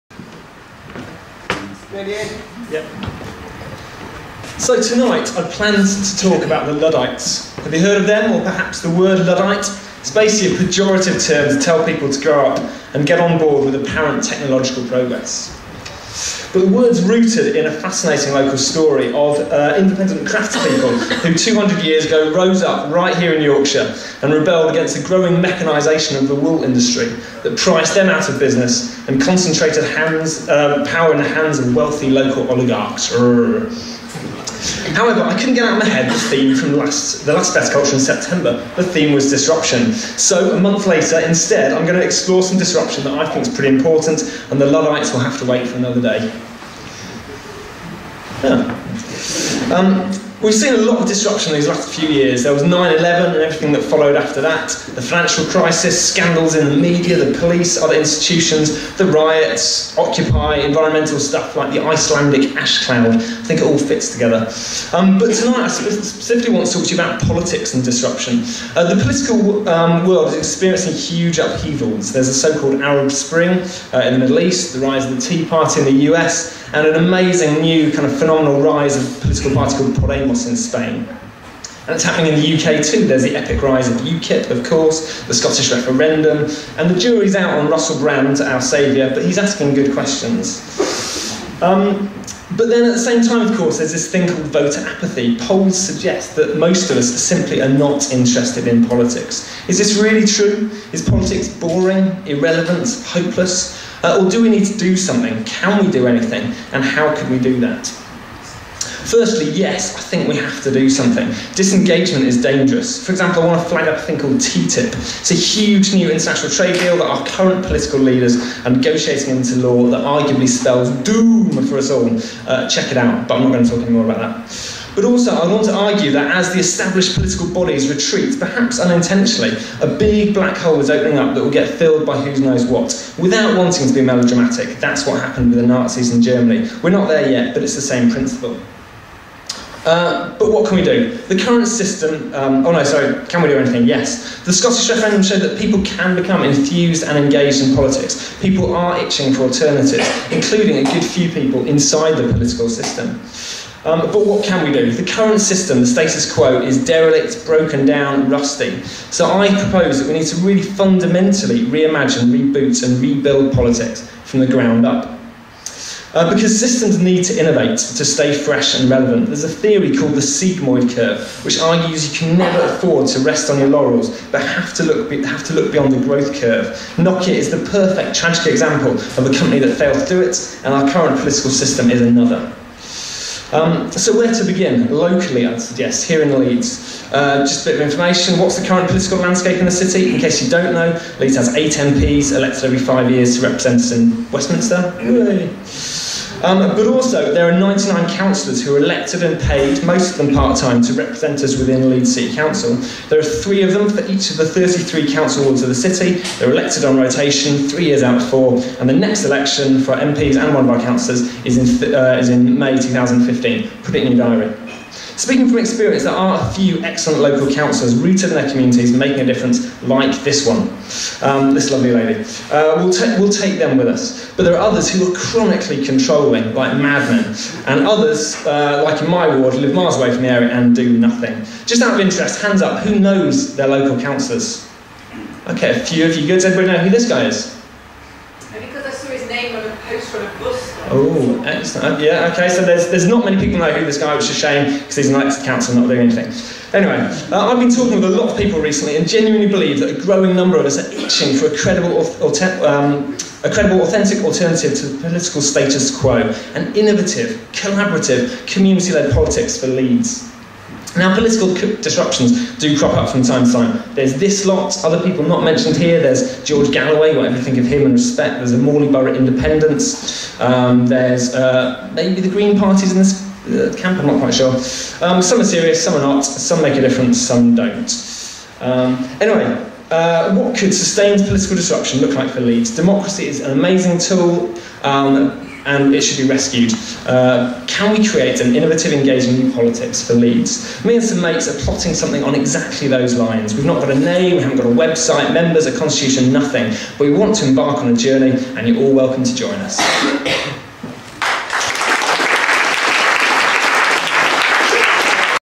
Ed Carlisle - Political Disruption
Ed discusses how our politics are going through a period of massive disruption. Who will fill the black hole in the power struggle and what can we do about it?